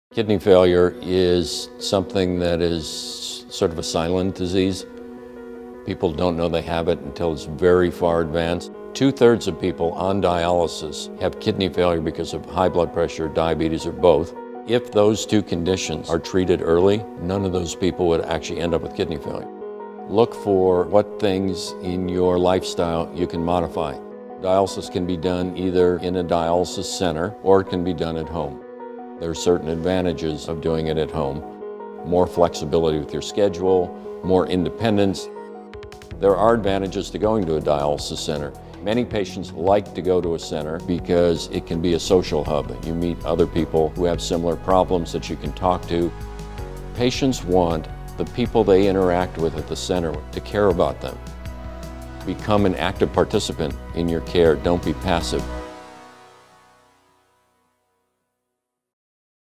Kidney Disease Education - Be
Kidney disease is often called a "silent disease," because it typically shows no symptoms. If people know more about the risks of kidney disease—such as diabetes and high blood pressure—it could help prevent or delay the condition. Hear from dialysis patients and kidney care experts about the importance of becoming kidney aware.